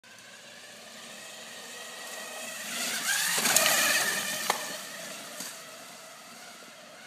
Electric mountain bike! 410 Cycles!
Exiting a fun trail! 410 Cycles electric fun!!